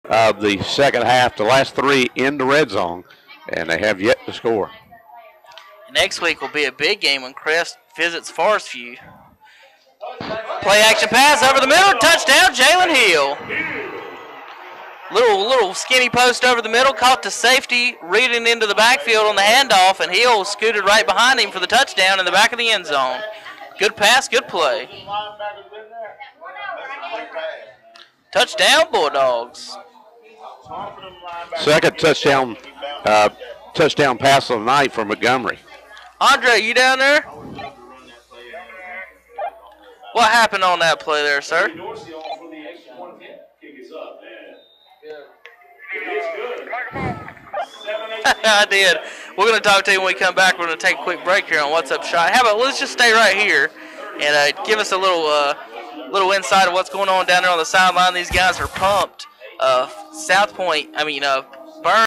Chris Montgomery 14yd TD pass to Jhalen Hill vs Q Foundation - 9/14/12
Burns Bulldogs defeat Q Foundation in Lawndale 41-0 - 9/14/12